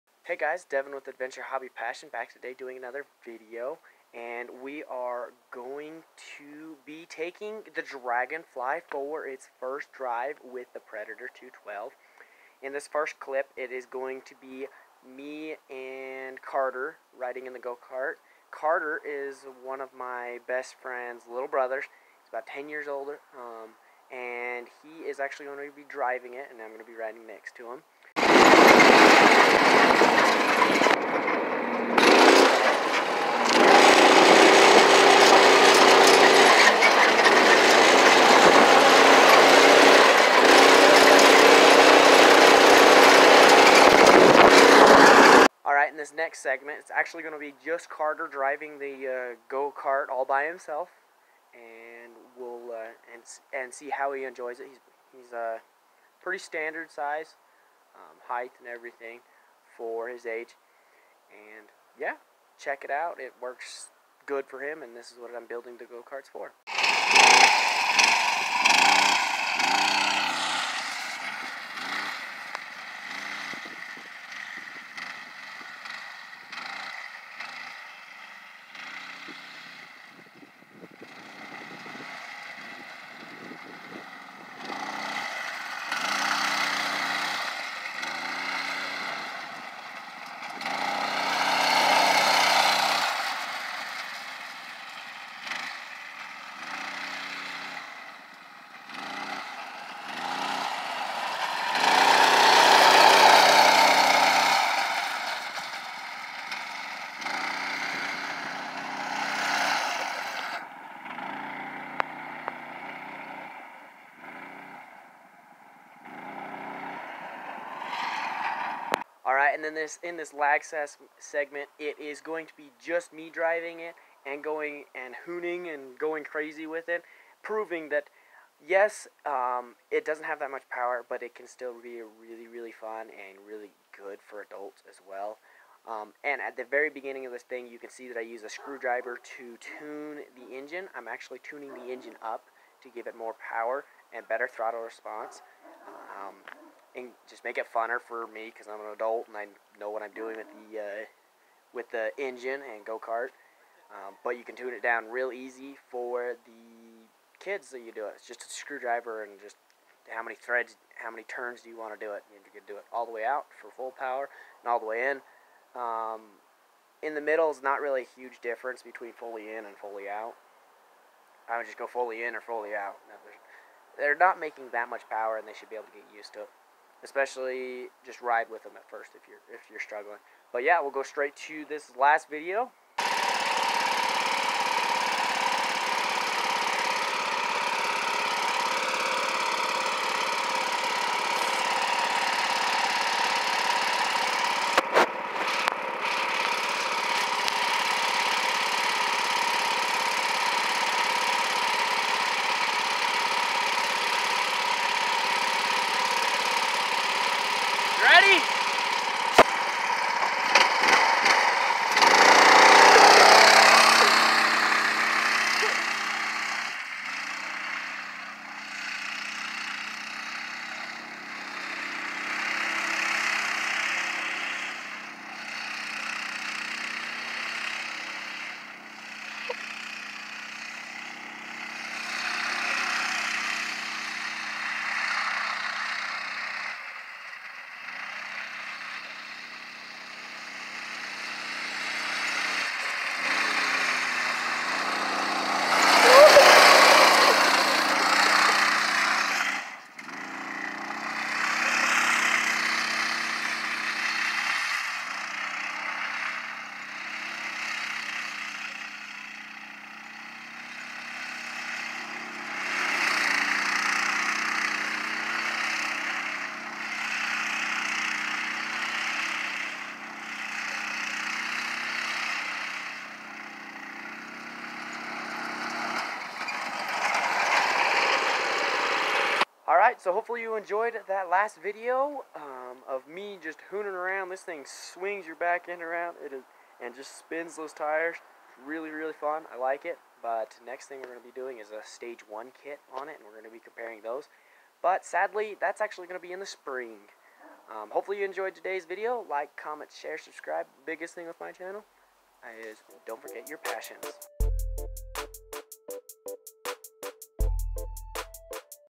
DRAGONFLY GO-CART FIRST DRIVE
In this video carter and I are able to enjoy the dragon fly gocart. He test drive with me in it and also with just him in it. at the end. I tune it up and give it a spin myself. the gocart really rips.